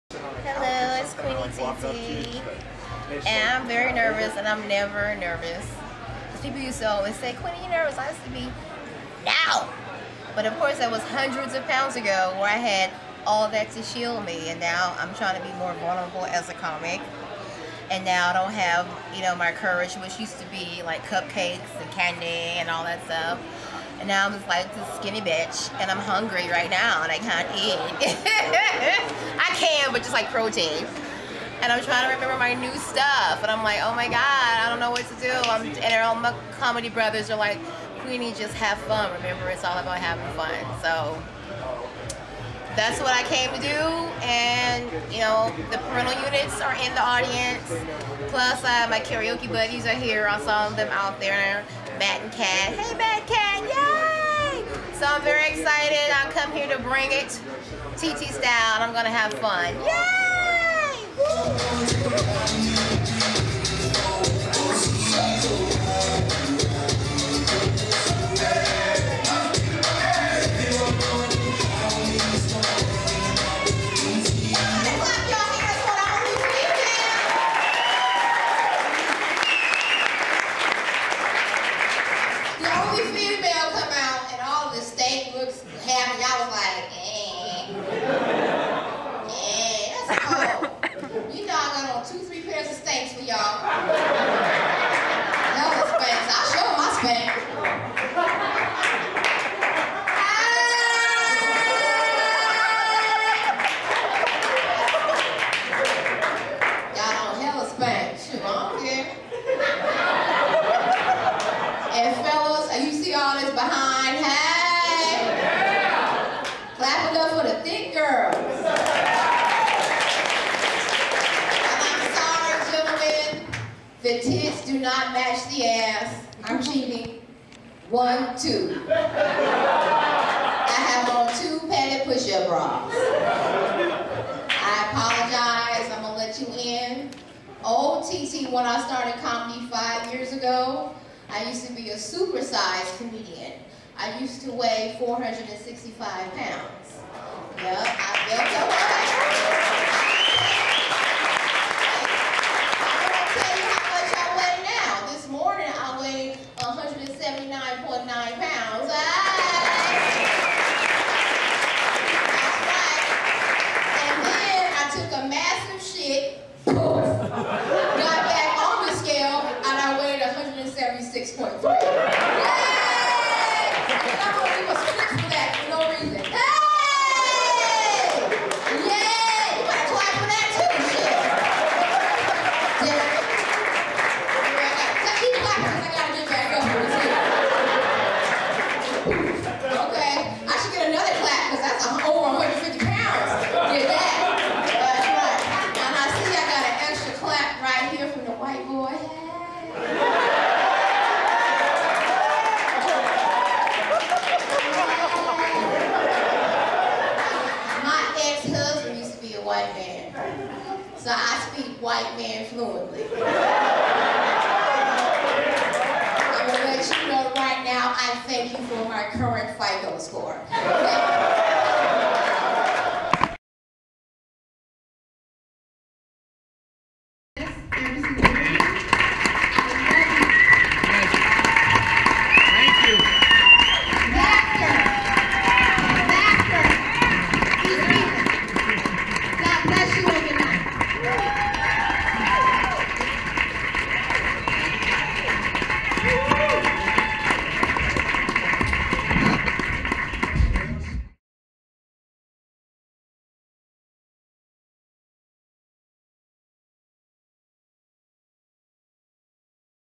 The Rebirth of Comedienne Queenie TT
After a 290 pound weight loss, America's Favorite Sister-Girl,  Comedienne Queenie TT is back on the mic with a new monologue called The Queenie Experience: a mini one woman show in 15 minutes or more! Queenie mixes traditional stand up comedy with storytelling, improv, and superb crowd work creating a unique experience for each individual audience she performs for. There is only one Comedienne Queenie TT. Her story is her brand and her unique, quirky, big energy style makes her absolutely irresistible. She's an inspirational charmer and funny too!